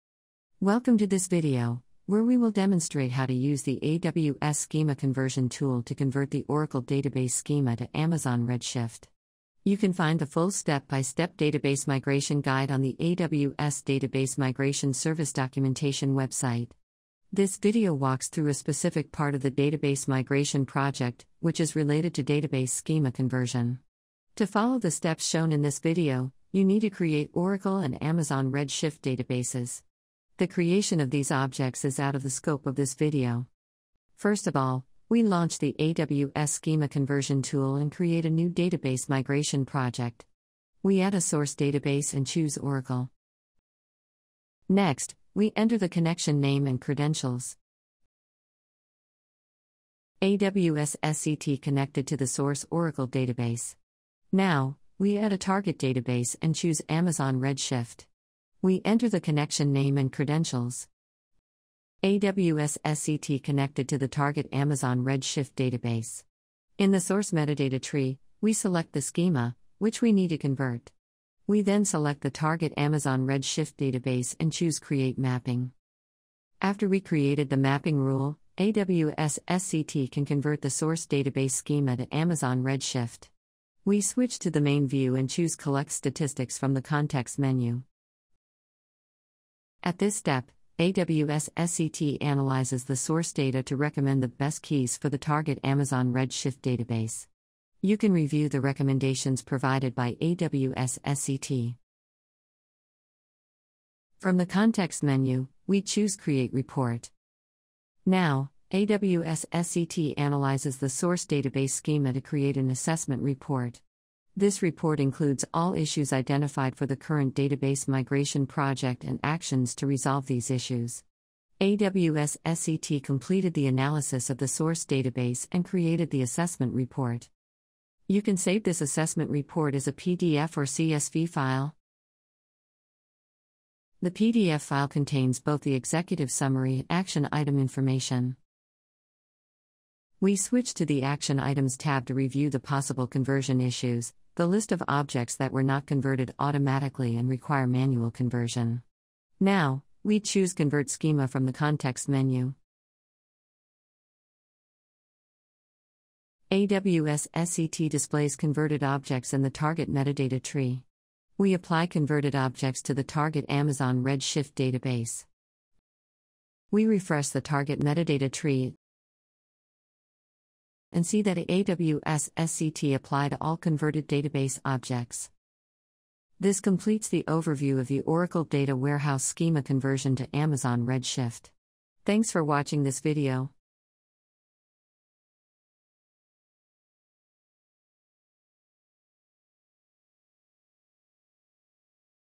Migrating an Amazon RDS for Oracle database to Amazon Redshift | Amazon Web Services
This walkthrough gets you started with heterogeneous database migration from Amazon RDS for Oracle to Amazon Redshift using AWS Database Migration Service (AWS DMS) and the AWS Schema Conversion Tool (AWS SCT).